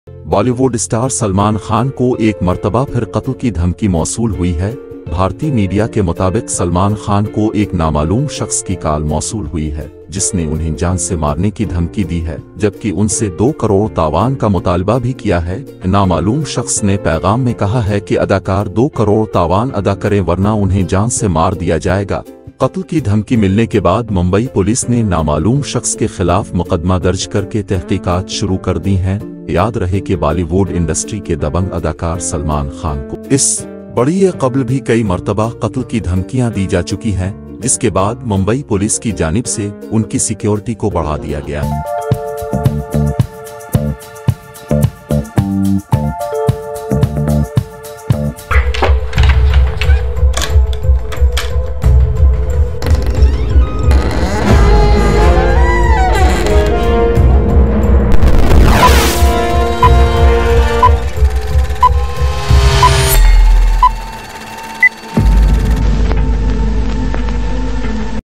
Salman Khan's LIFE UNDER THREAT Again! | HM Entertainment
Salman Khan's life has once again been threatened by an unknown entity, demanding a staggering ransom of crores of rupees. The Bollywood superstar has been at the receiving end of numerous death threats in the past, but this latest demand has sent shockwaves throughout the industry. In this video, we delve into the details of this shocking incident and explore the possible motives behind this daring ransom demand. From the police investigation to the star's own security measures, we bring you all the latest updates on this developing story. Is Salman Khan's life really in danger, or is this just another empty threat? Watch to find out!